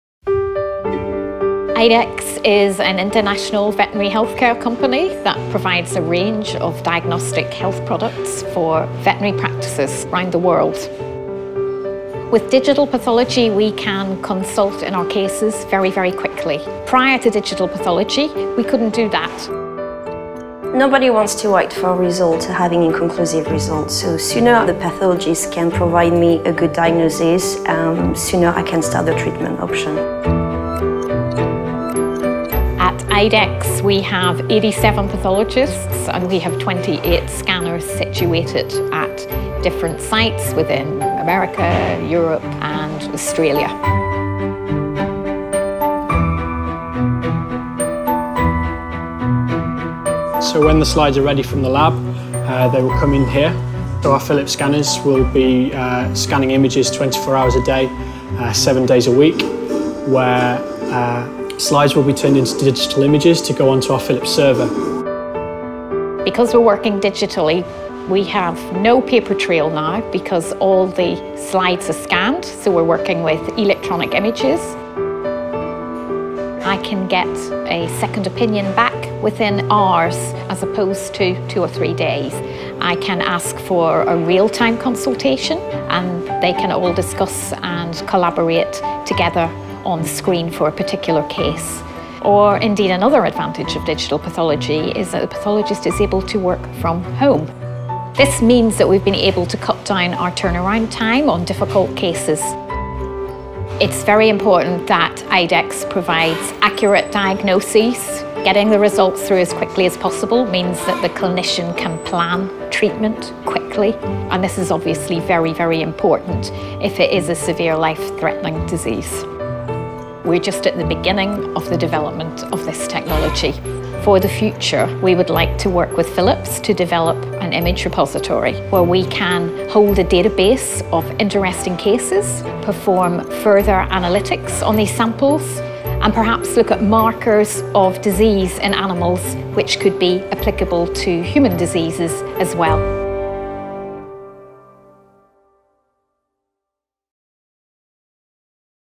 Idexx reference laboratories' global digital pathology network
IDEXX is an international veterinarian healthcare company that provides a range of diagnostic health products for veterinary practices around the world.
With digital pathology we can consult in our cases very, very quickly. Prior to digital pathology we could not do that.
Nobody wants to wait for a result or having inconclusive results. The sooner pathologists can provide me a good diagnosis, the sooner I can start the treatment option.
At IDEXX, we have 87 pathologists and we have 28 scanners situated at different sites, within America, Europe and Australia.
So when the slides are ready from the lab, they will come in here. There are Philips scanners that will be scanning images 24 hours a day, 7 days a week. Where slides will be turned into digital images to go onto our Philips server.
Because we are working digitally, we have no paper trail now because all the slides are scanned, so we are working with electronic images. I can get a second opinion back within hours as imposed to 2 or 3 days. I can ask for a real-time consultation. And they can all discuss and collaborate together on-screen for a particular case. Or indeed another advantage of digital pathology, is that the pathologists is able to work from home. This means that we are able to cut down our turnaround time on difficult cases. It is very important that IDEXX provides accurate diagnosis, getting the results through as quickly as possible, means that the clinician can plan treatment quickly and this is obviously very, very important if it is a severe life threatening disease.
We are just at the beginning of the development of this technology. For the future we would like to work with Philips to develop an image repository. Where we can build a database of interesting cases, perform further analytics on the samples and perhaps good markers of disease in animals that could be applicable to human diseases as well.